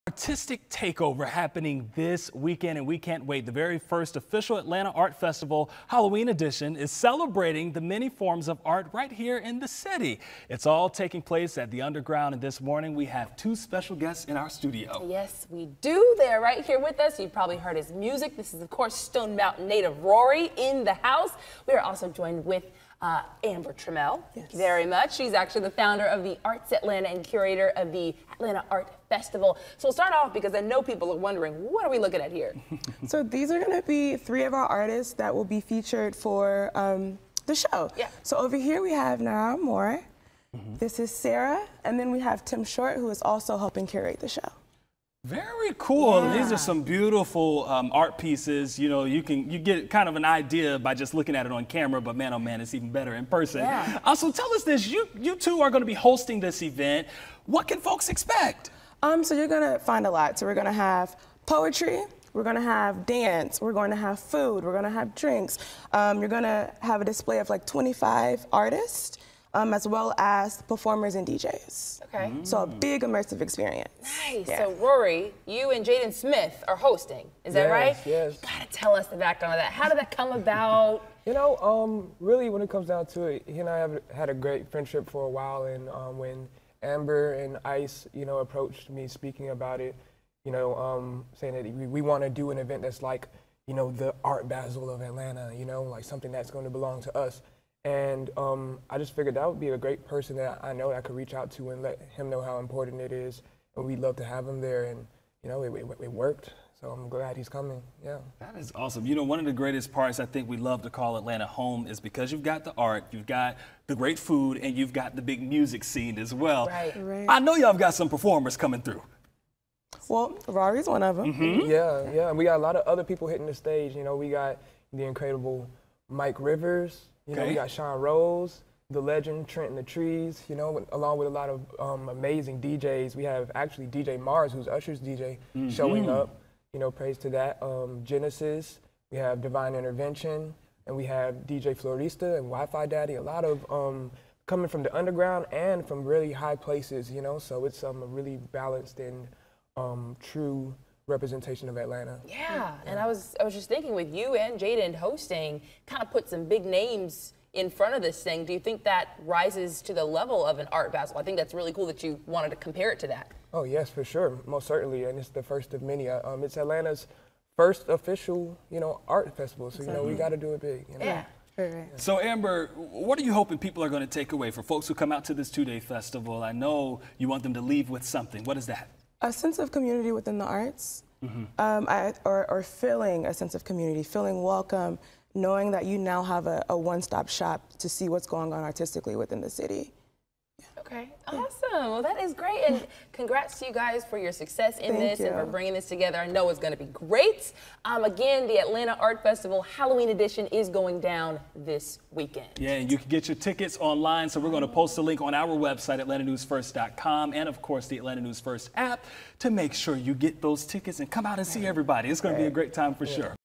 INTERVIEW: Atlanta Art Festival comes to Underground Atlanta
The Atlanta Art Festival is coming to Underground Atlanta this weekend! Full story